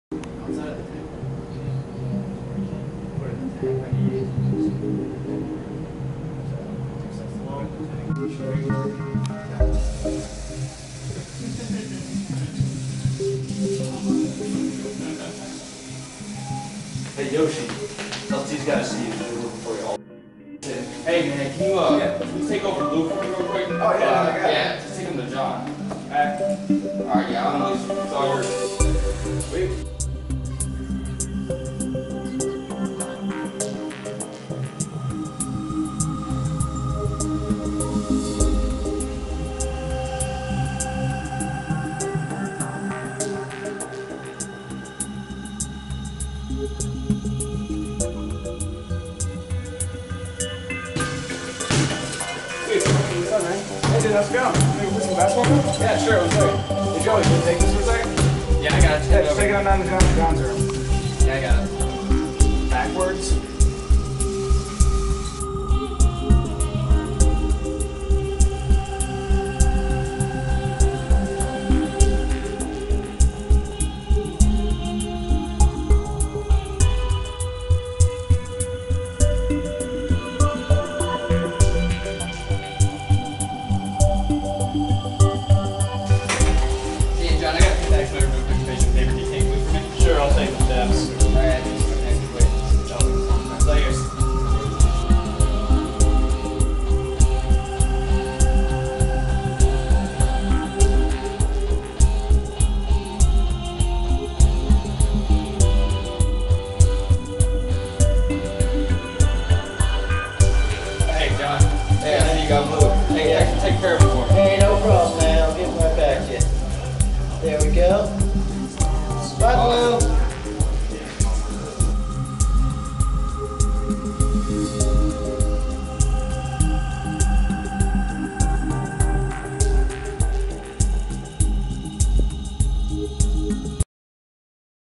BlueBot Movie.mpg
A Bluetooth controlled robot for my Senior Design Project USNA 2011 Computer Engineering.